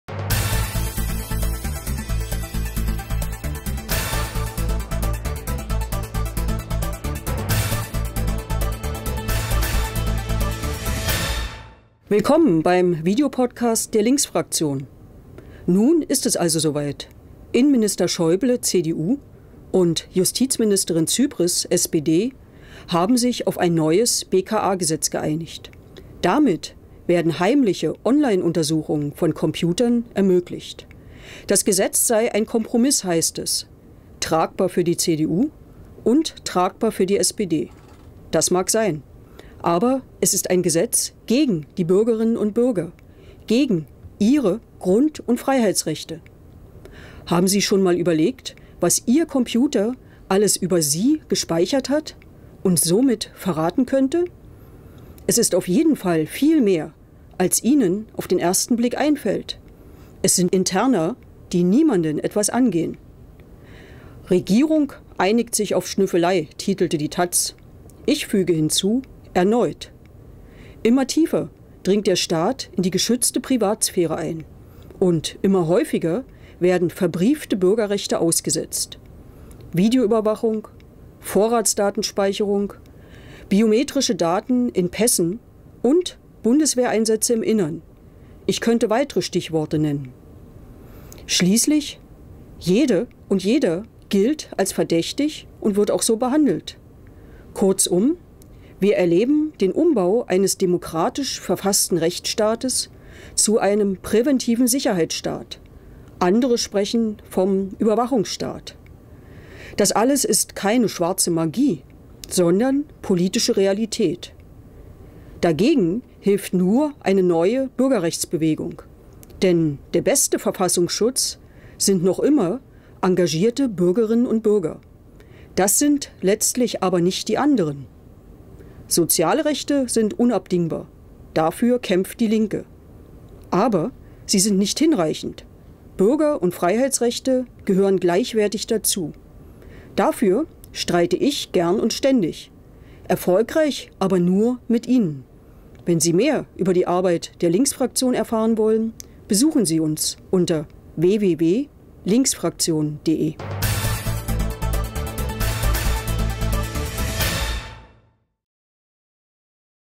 DIE LINKE: Petra Pau zur Online-Durchsuchung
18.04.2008 - Videoüberwachung, Vorratsdatenspeicherung, biometrische Daten in Pässen und Bundeswehreinsätze im Inneren: Immer tiefer dringt der Staat in die geschützte Privatsphäre ein. Immer häufiger werden verbriefte Bürgerrechte ausgesetzt.